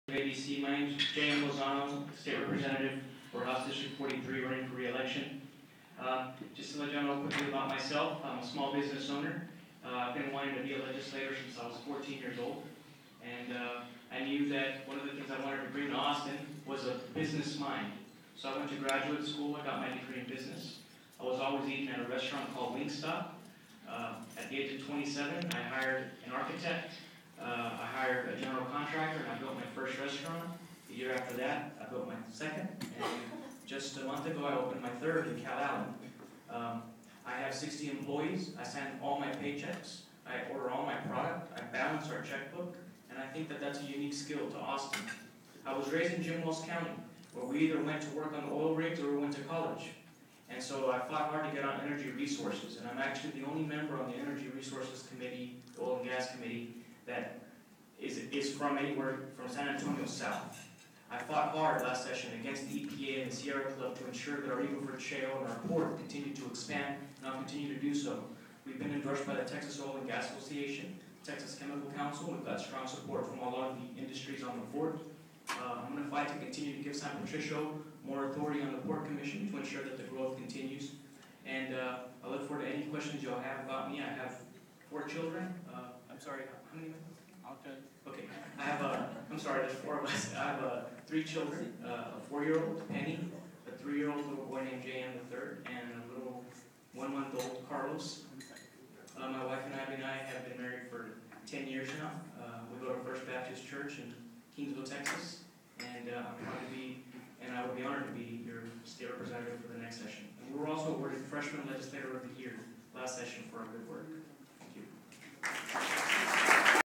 State District 43 candidate J.M. Lozano
State Rep. Lozano spoke before members and guests of the Coastal Bend chapter of Associated Builders and Contractors Inc.